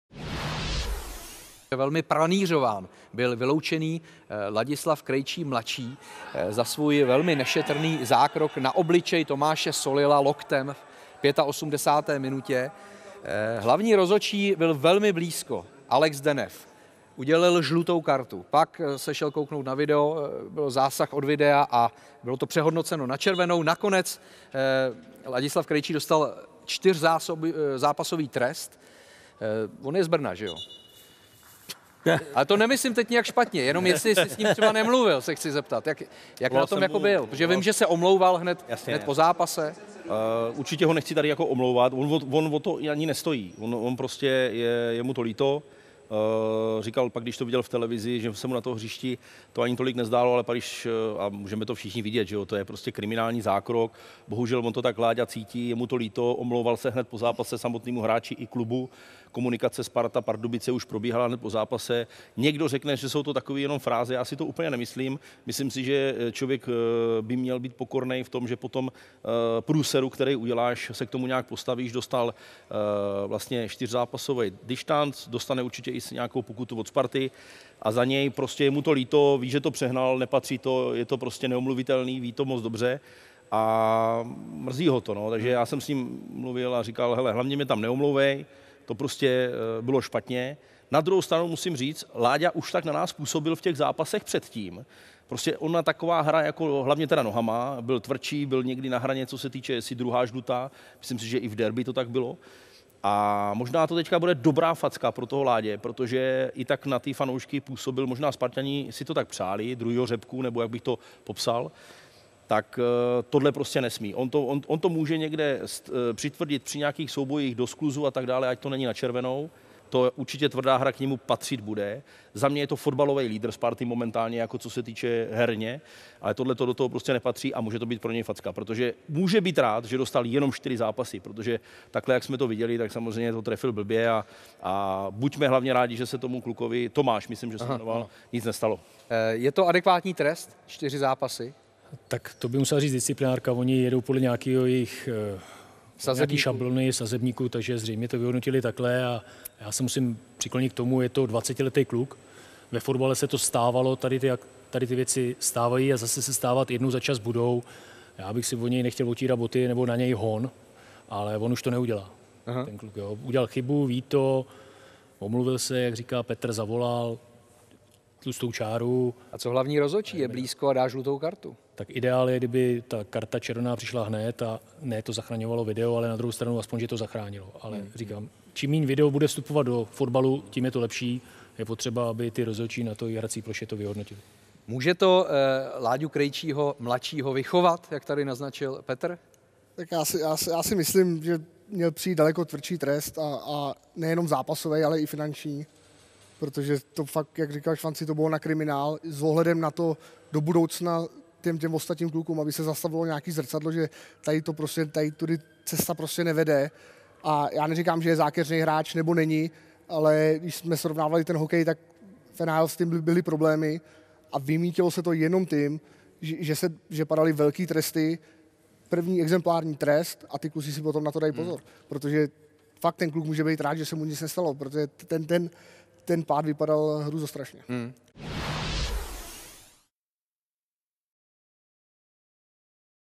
TIKI-TAKA: Krejčího loket a čtyřzápasová stopka? Je to neomluvitelné
V poslední TIKI-TAKA roku jsme se vrátili i k zákroku Ladislava Krejčího mladšího na Tomáše Solila. Za loket v obličeji vyfasoval na hřišti červenou a navíc ještě čtyřzápasový flastr. "Mrzí ho to a ví, že to přehnal. Je to neomluvitelné a možná to pro něj bude dobrá facka," myslí si Petr Švancara.